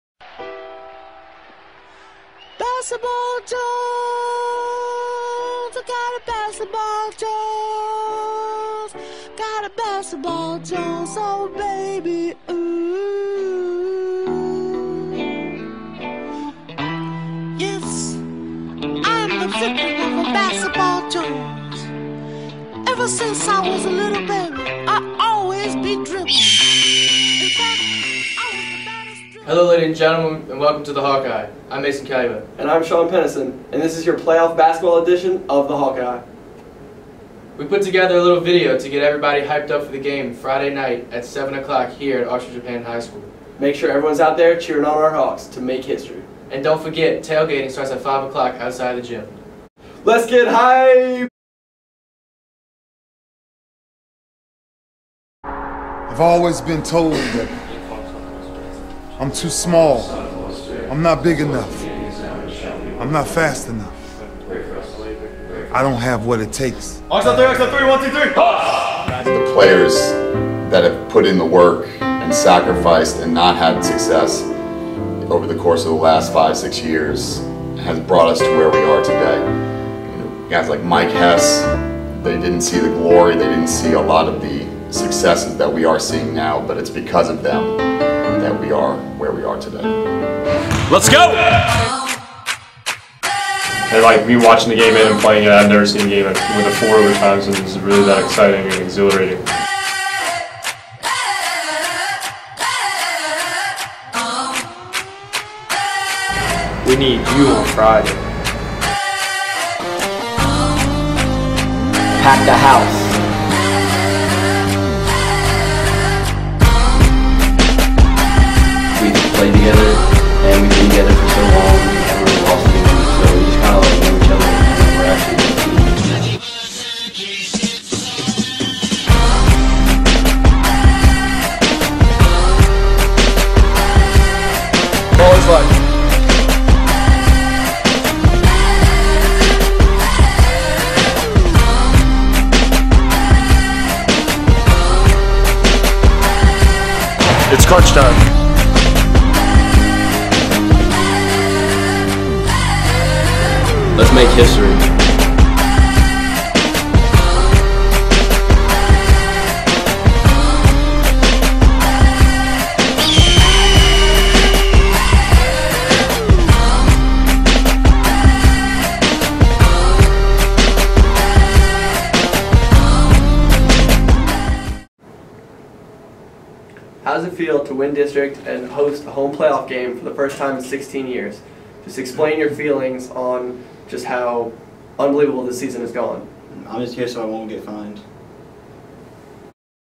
Archbishop Hannan Basketball Hype Video
Hype video for Friday 2/27 Hannan vs. Vinton at the Hawk's Nest